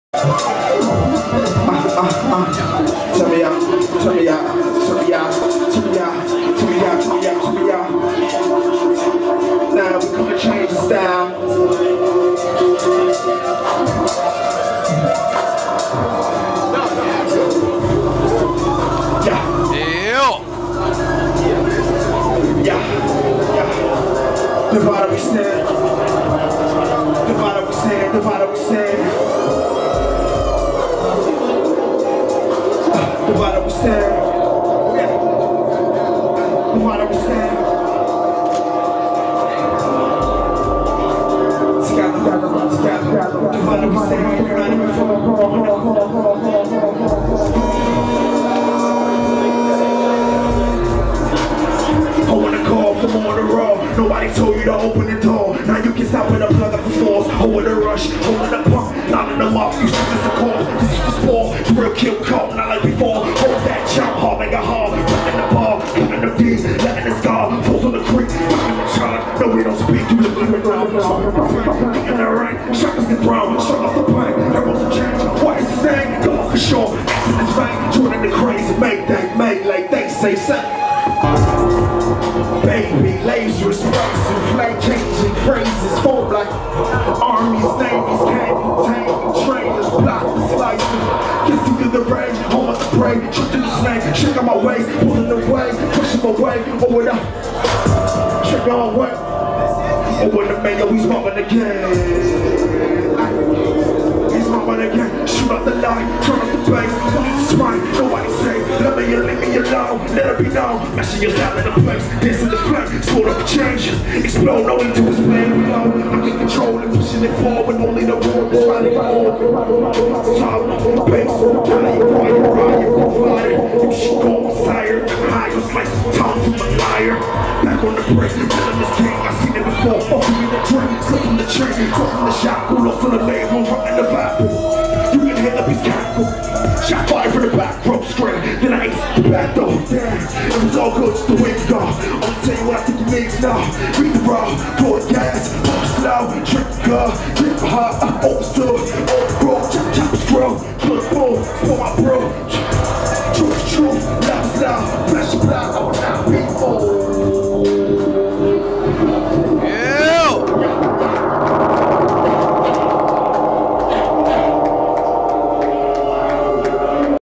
Elucid - @ 4th Annual Yule Prog, Southpaw, NYC
Elucid performing @ 4th Annual Yule Prog, Southpaw, NYC on December 14th, 2010.